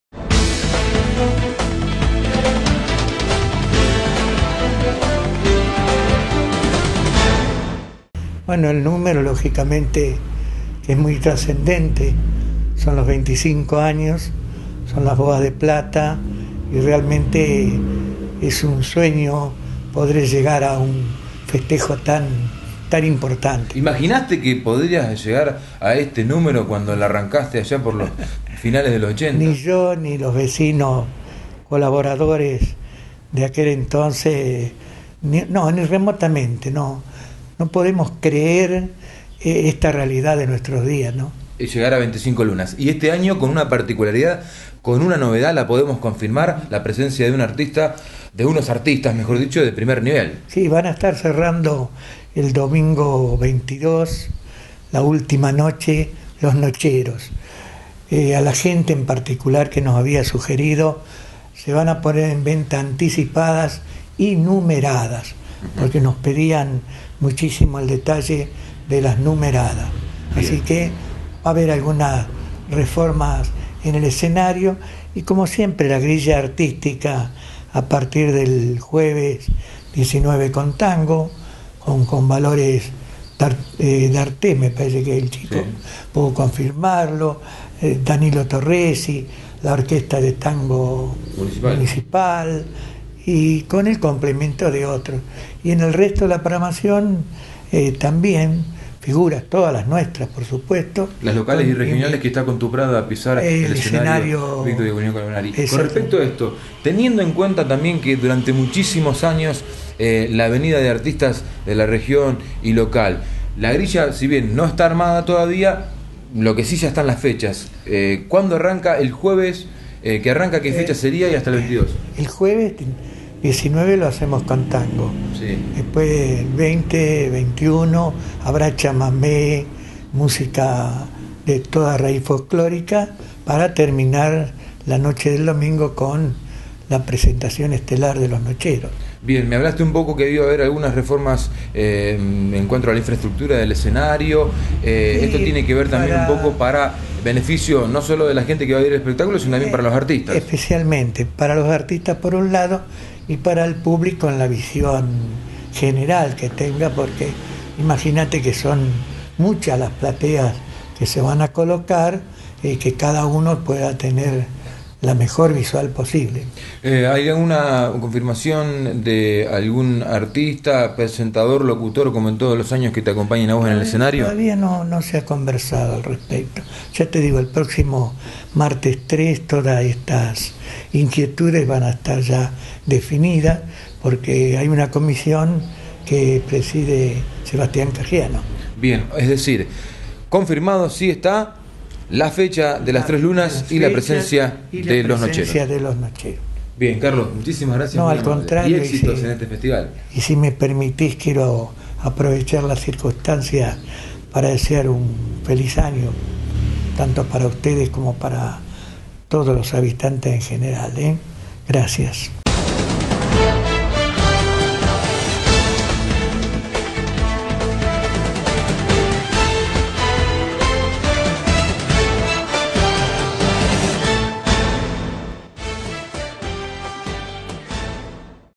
FESTIVAL FOLCLÓRICO LAS TRES LUNAS
La comisión organizadora del próxima festival folclorico " Las Tres Lunas" ya puso fecha al próximo evento que tendrá lugar desde el 19 al 22 de enero de del año próximo. Además, ya confirmaron que contarán con la participación de "Los Nocheros. Hoy hablamos con Carlos Faúl sobre el tema.